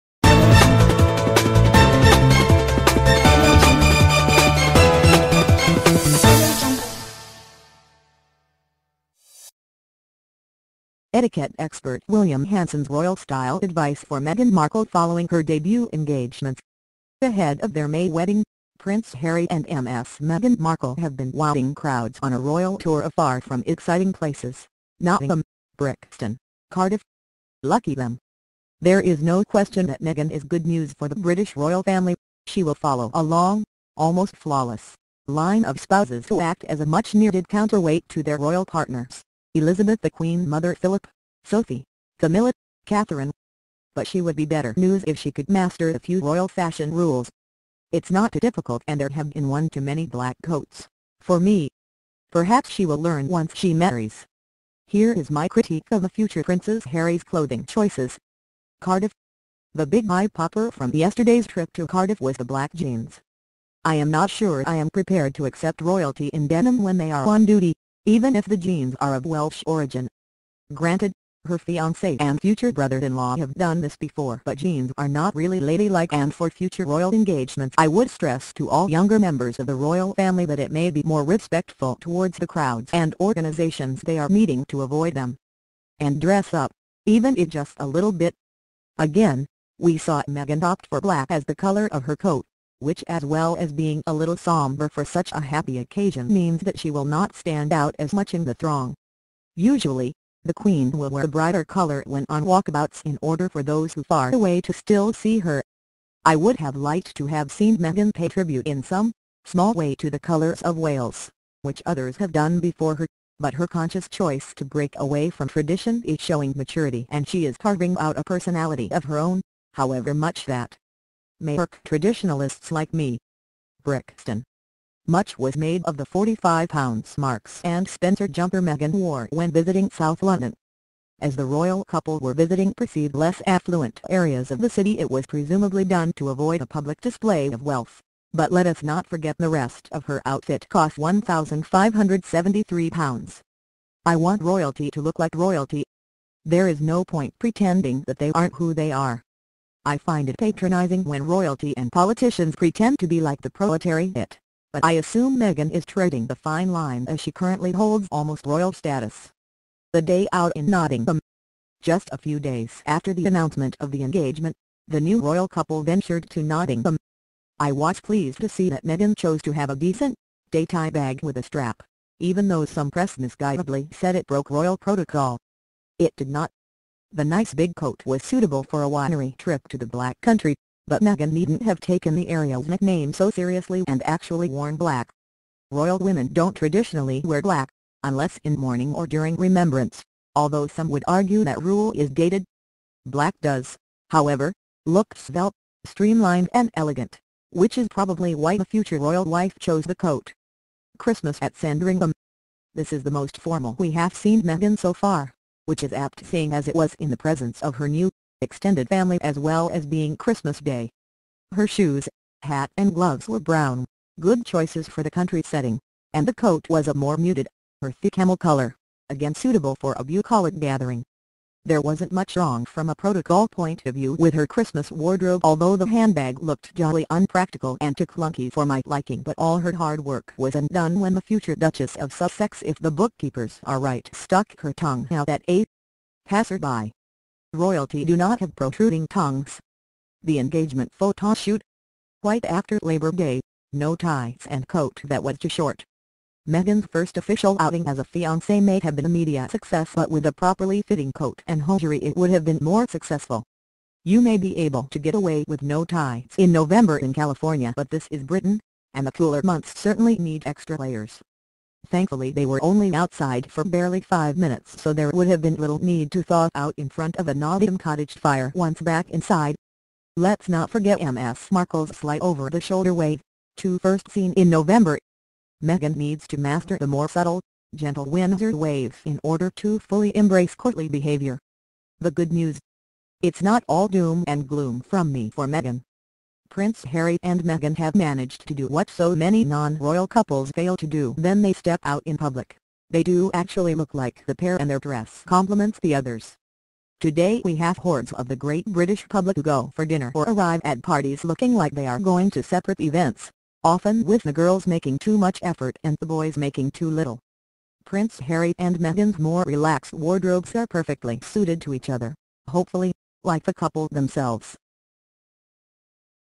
Etiquette expert William Hanson’s royal style advice for Meghan Markle following her engagements
Ahead of their May wedding, Prince Harry and Ms Meghan Markle have been wowing crowds on a royal tour of far from exciting places: Nottingham, Brixton, Cardiff. Lucky them